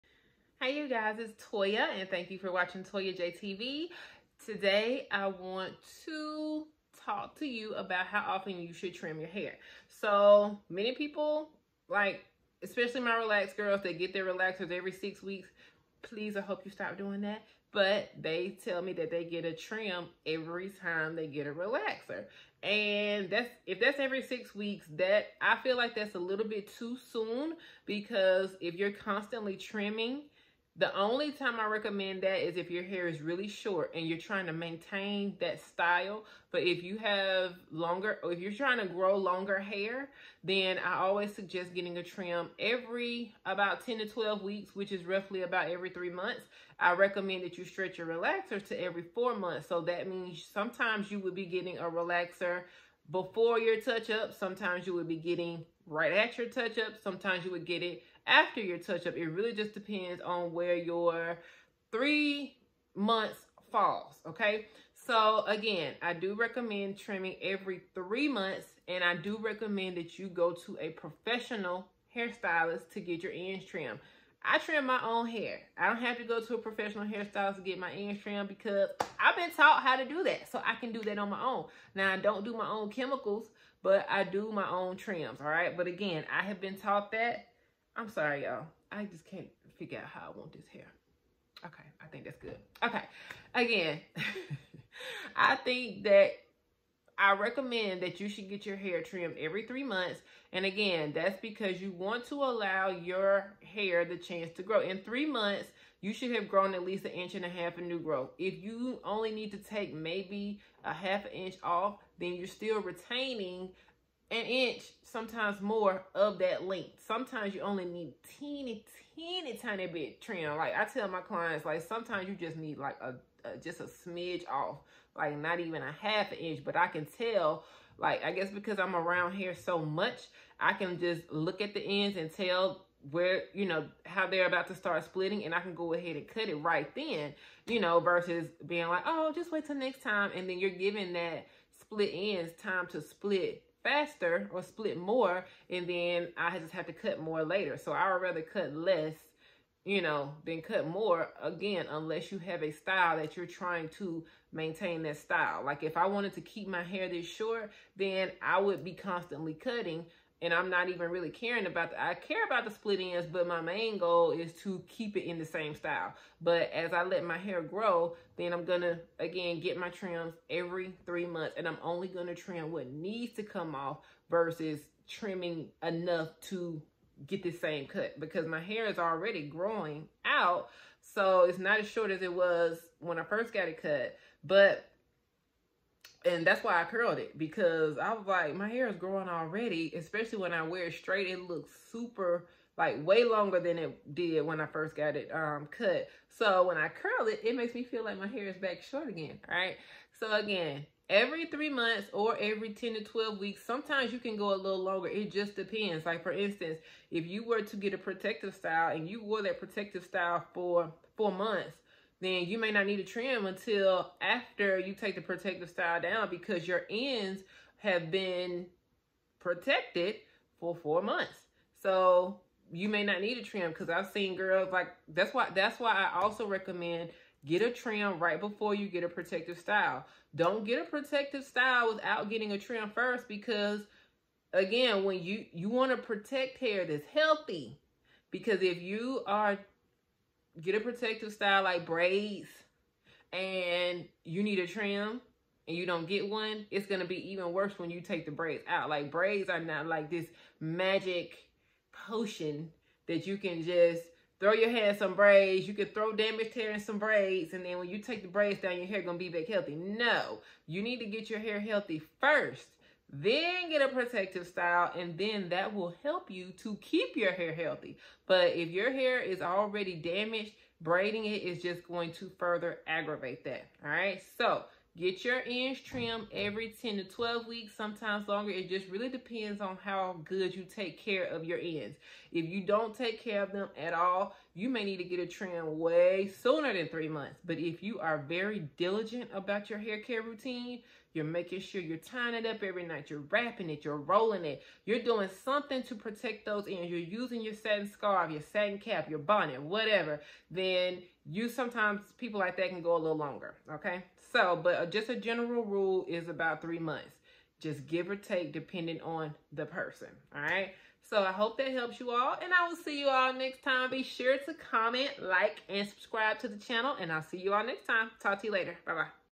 How often should I trim my hair?
Do you want to know how often you should trim your hair? Whether you are natural it relaxed the same rules apply! Check out this video to find out more.